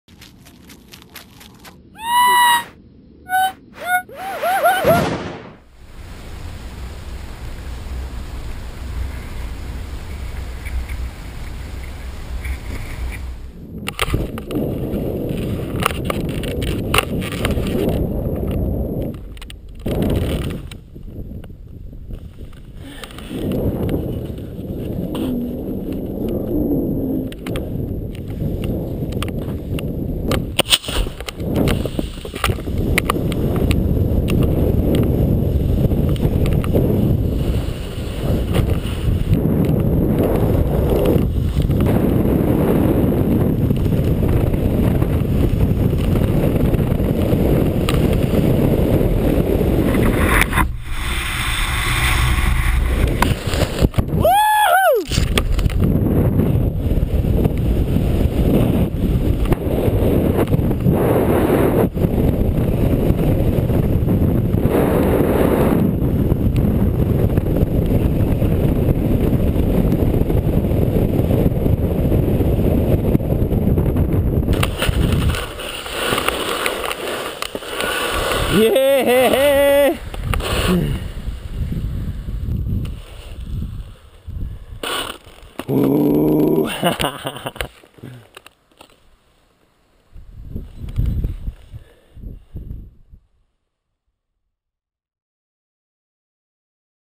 Mt Hood: Mississippi Head Ski BASE - WIDSIX presents The Super Frenchie Diaries with Matthias Giraud
Mt Hood: Mississippi Head Ski BASE - WIDSIX presents The Super Frenchie Diaries with Matthias Giraud episode #9

In this edit, we enjoy some archive footage of Super Frenchie enjoying some spring skiing... going back to the first cliff he ever ski BASE jumped off of: Mississippi Head.